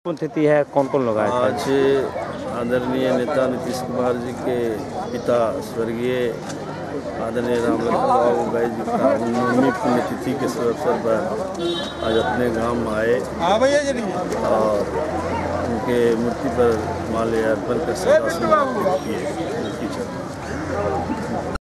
PNA NEWS AGENCY
From -- Nalanda -- नालंदा जिले के कल्याण विगहा गांव में आज मुख्यमंत्री नीतीश कुमार पहुंच अपने पिता स्वः कवी रामराज सिंह बैध के नौवें पुण्य तिथि के अवसर पर पुष्प अर्पित की  इस दौरान उन्होंने आम जनता से भी मुखातिब हुए-- Byte -- संजयकांत सिंह जदयू नेता